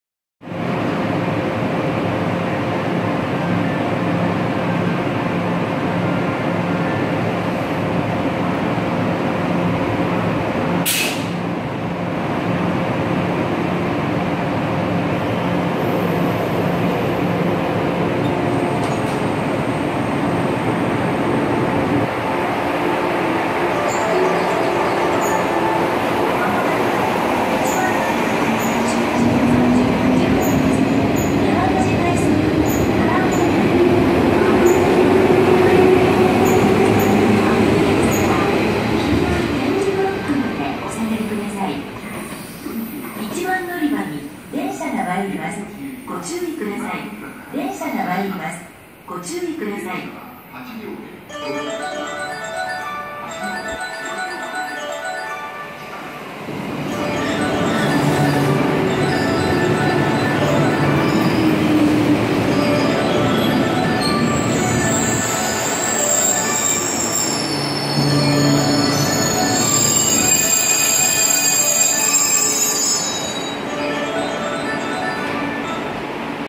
JR西日本 大阪環状線 103系 外回り 発車 / 221系 大和路快速 加茂行き 寺田町駅 到着
JR 서일본 오사카환상선 103계 외선순환 열차 발차/ 221계 야마토지쾌속 가모행 열차 테라다초역 진입

JR Nishi - Nihon Osaka Loop Line 103 Series Outer Circle Line train depart and 221 Series Train bound for Kamo arriving at Teradacho Stn